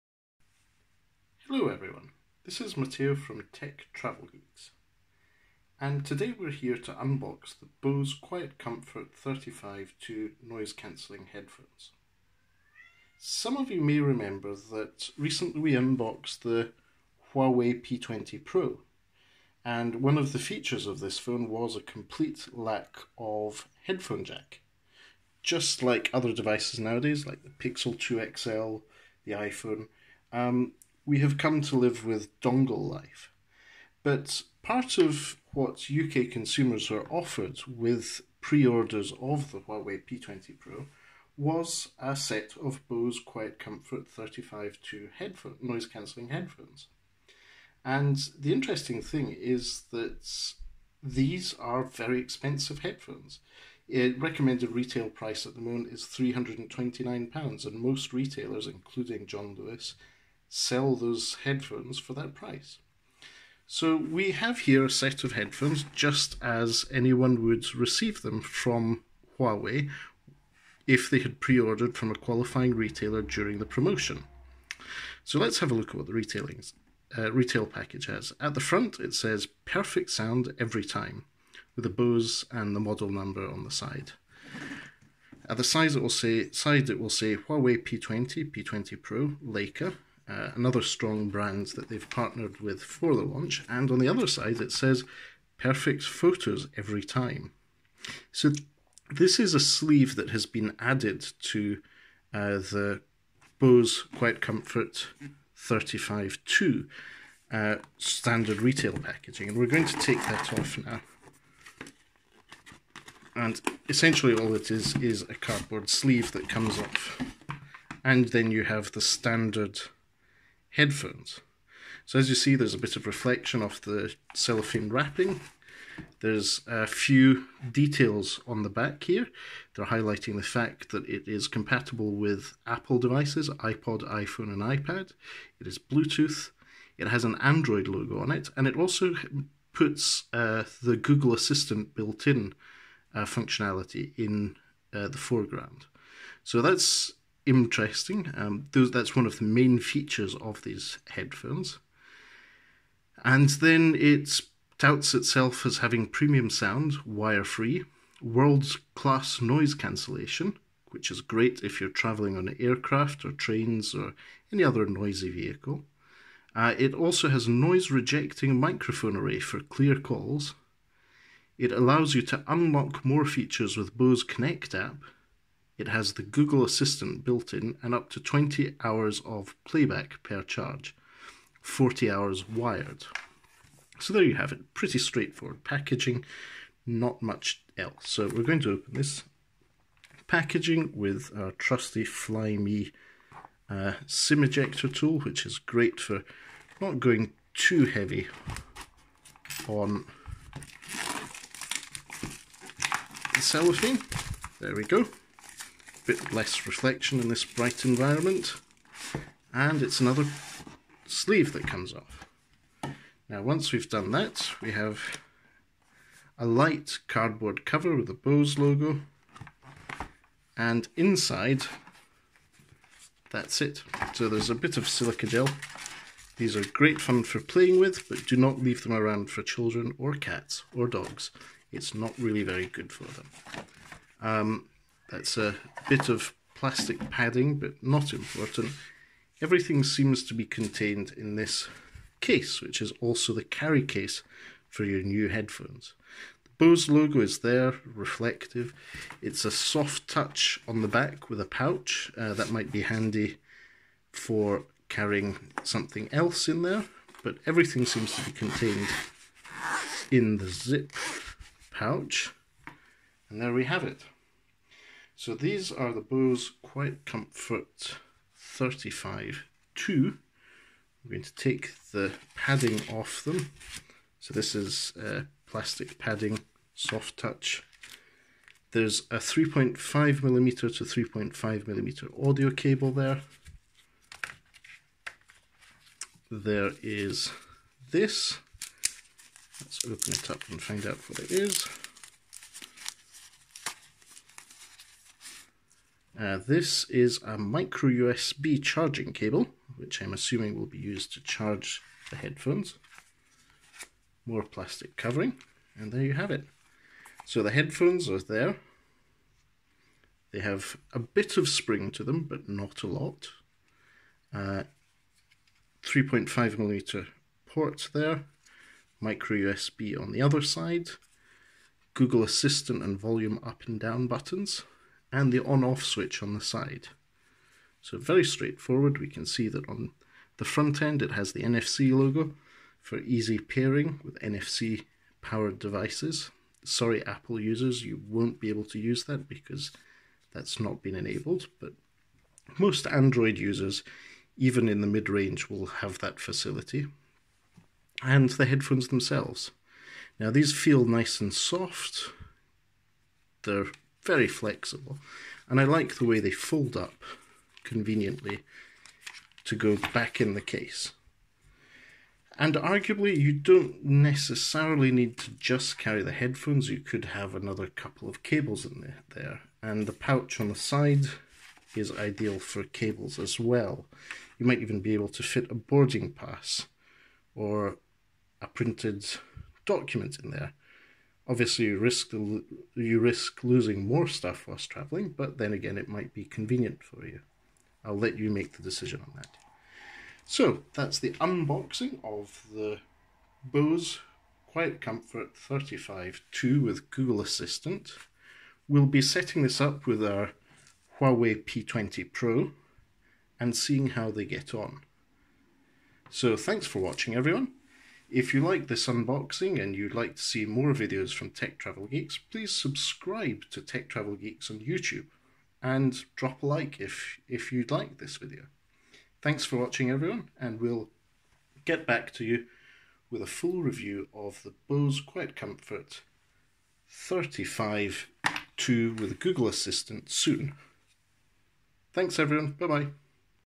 Bose QUIETCOMFORT 35 II Unboxing (Huawei P20 Pro UK pre-order offer)
Matteo unboxes the Bose QUIETCOMFORT 35 II wireless headphones with Google Assistant. These Bose QC 35 II were part of the Huawei P20 Pro pre-order offer in the UK.

Bose QuietComfort 35 II Noise Cancelling Bluetooth Headphones available to purchase on amazon

Shot on Honor 10.